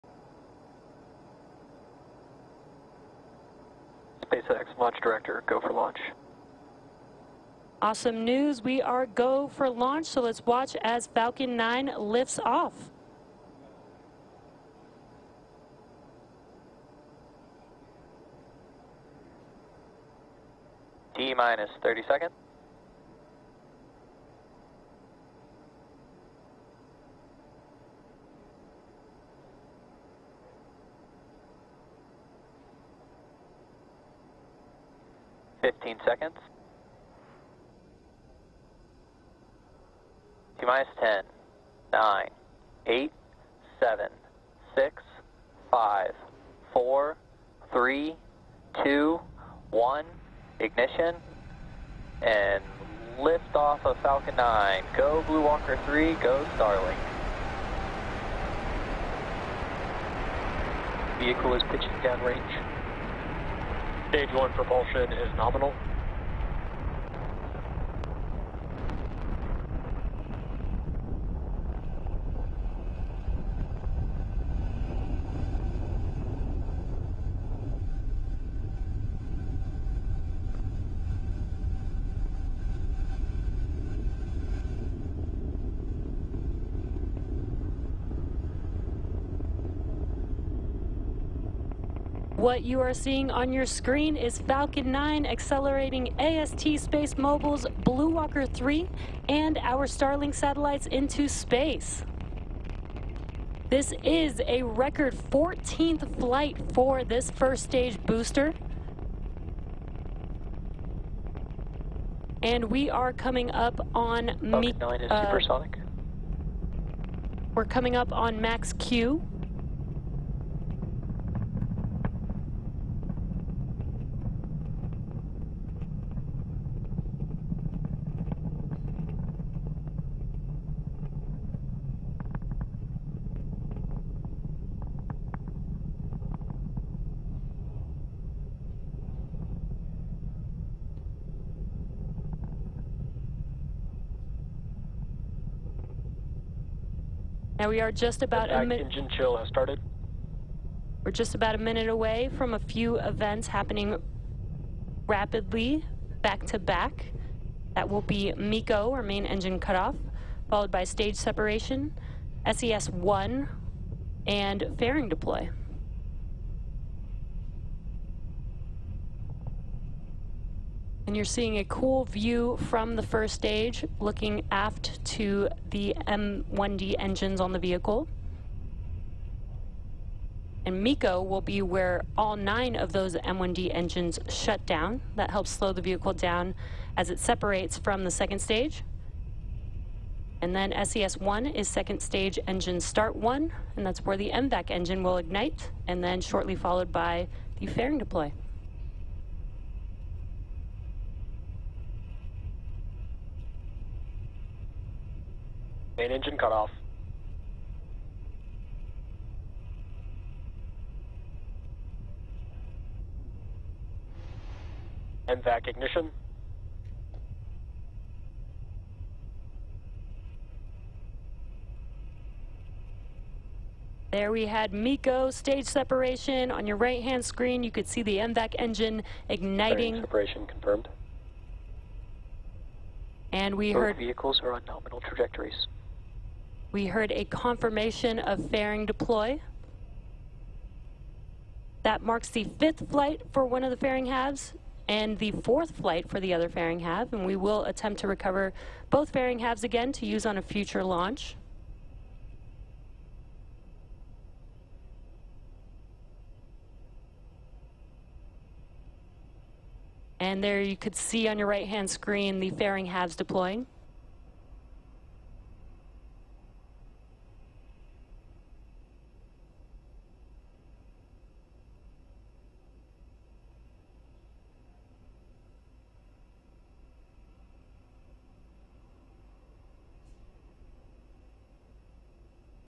SpaceX launches Falcon 9 out of Cape Canaveral | Sept. 10 2022
SpaceX Falcon 9 launches out of Cape Canaveral at 9:20 p.m. Saturday, September 10.